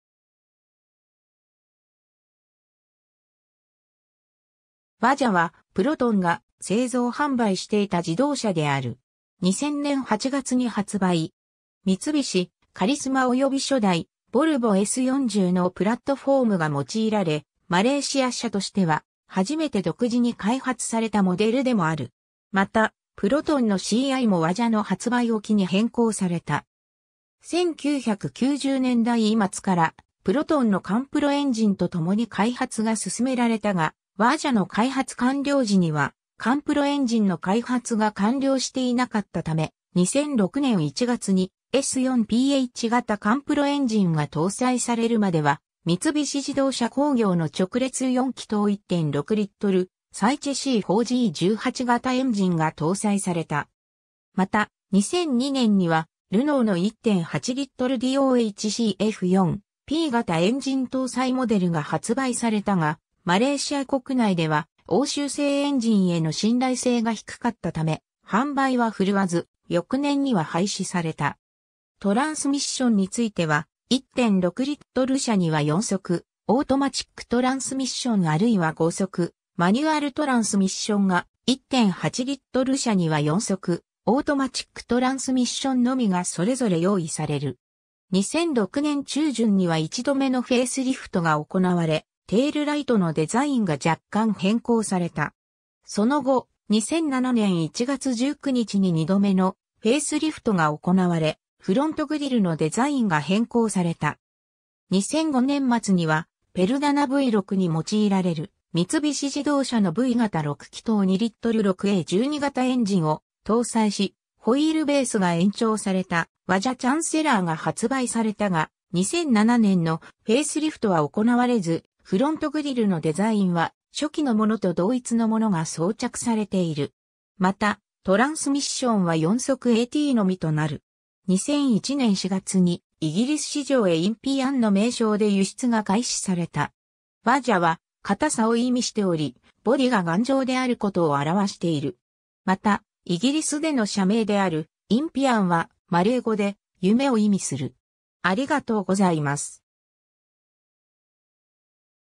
プロトン・ワジャ
ワジャ (WAJA)は、プロトンが製造販売していた自動車である。
 2000年8月に発売。
三菱・カリスマおよび初代ボルボ・S40のプラットフォームが用いられ、マレーシア車としては初めて独自に開発されたモデルでもある。
また、プロトンのCIもワジャの発売を機に変更された。
 1990年代末からプロトンのカンプロエンジンと共に開発が進められたが、ワジャの開発完了時にはカンプロエンジンの開発が完了していなかったため、2006年1月にS4PH型カンプロエンジンが搭載されるまでは三菱自動車工業の直列4気筒 1.6L SOHC 4G18型エンジンが搭載された。
また、2002年にはルノーの1.8L DOHC F4P型エンジン搭載モデルが発売されたが、マレーシア国内では欧州製エンジンへの信頼性が低かったため、販売は振るわず、翌年には廃止された。
トランスミッションについては、1.6L車には4速オートマチックトランスミッションあるいは5速マニュアルトランスミッションが、1.8L車には4速オートマチックトランスミッションのみがそれぞれ用意される。
 2006年中旬には1度目のフェイスリフトが行われ、テールライトのデザインが若干変更された。
その後、2007年1月19日に2度目のフェイスリフトが行われ、フロントグリルのデザインが変更された。
 2005年末にはペルダナV6に用いられる三菱自動車のV型6気筒 2L 6A12型エンジンを搭載し、ホイールベースが延長されたワジャ チャンセラー (Waja Chancellor) が発売されたが、2007年のフェイスリフトは行われず、フロントグリルのデザインは初期のものと同一のものが装着されている。
また、トランスミッションは4速ATのみとなる。
 2001年4月にイギリス市場へインピアンの名称で輸出が開始された。
 「Waja」は「硬さ」を意味しており、ボディが頑丈であることを表している。
また、イギリスでの車名である「Impian」はマレー語で「夢」を意味する。